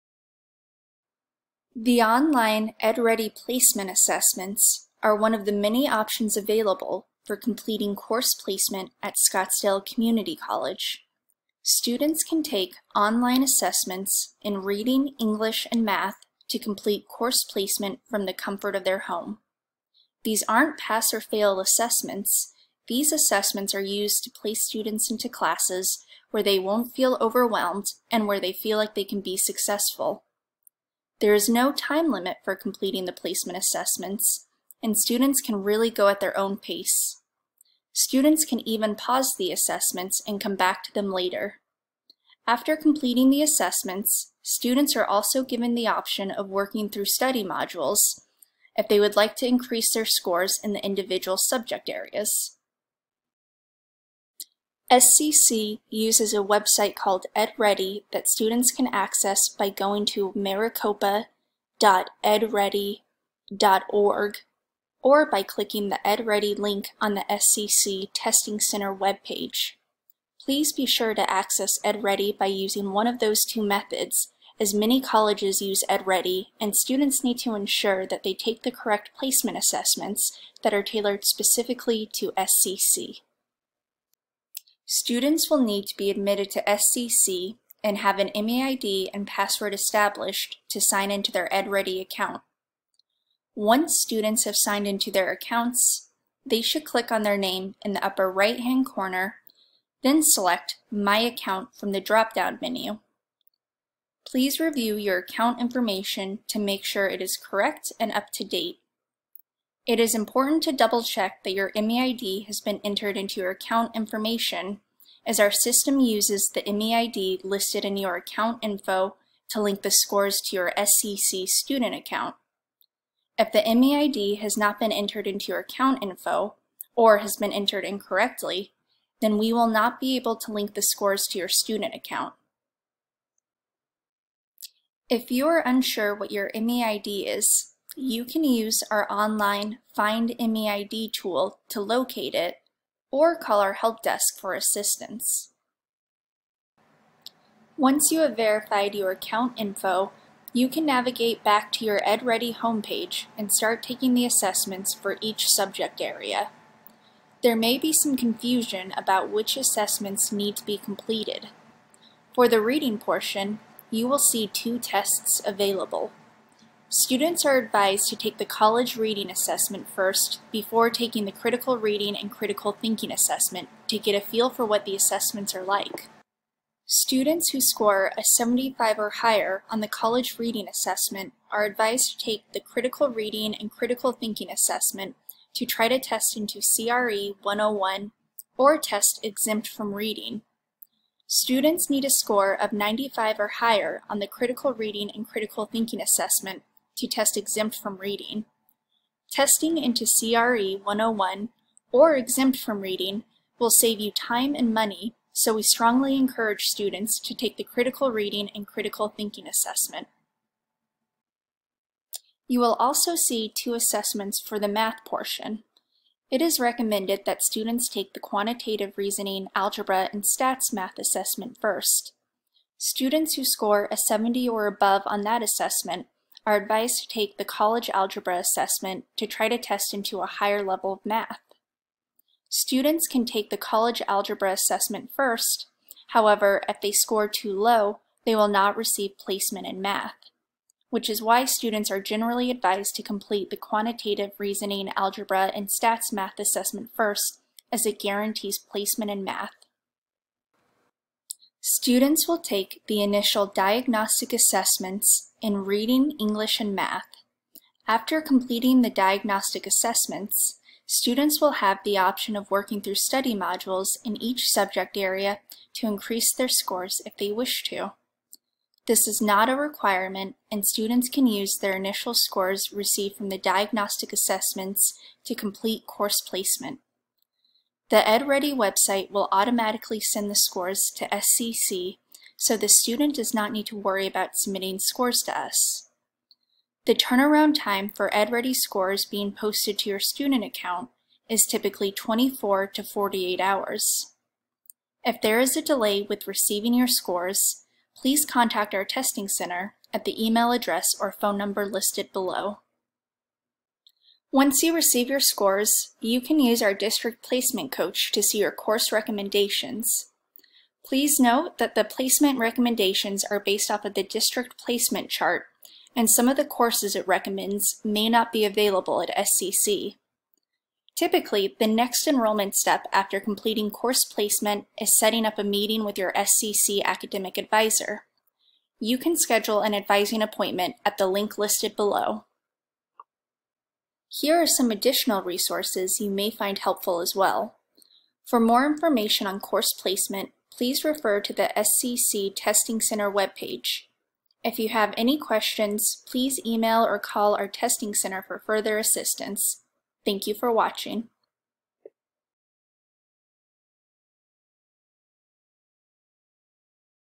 Course Placement through EdReady
Online Placement Assessments help future students determine course placement in reading, English, and math. These assessments are not pass/fail, and there is no time limit. Students have the option of completing study modules to potentially increase their scores in various subject areas.

to find the EdReady assessment link. We look forward to welcoming you to Scottsdale Community College!